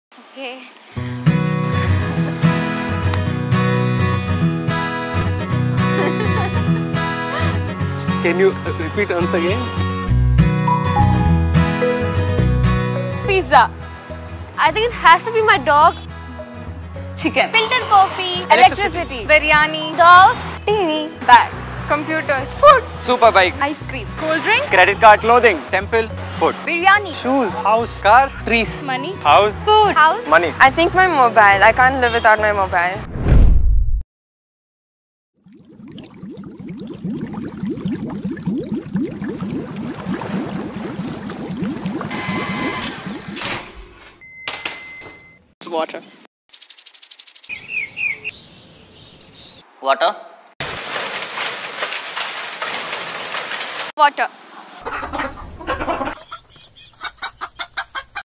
Discover the Most Important Thing in Your Life with this Thought-Provoking Video!
Are you feeling lost or unsure about what truly matters in your life? This thought-provoking video will help you discover the most important thing in your life. Through a series of powerful images and inspiring quotes, the video invites you to reflect on your values, priorities, and goals. It encourages you to question what truly brings you happiness, fulfillment, and purpose. The video highlights the importance of love, relationships, personal growth, and making a positive impact on others and the world around us. Whether you're going through a difficult time or simply looking to gain a new perspective on life, this video is a powerful tool that can help you gain clarity and inspiration. So join us on this journey of self-discovery and find out what truly matters in your life!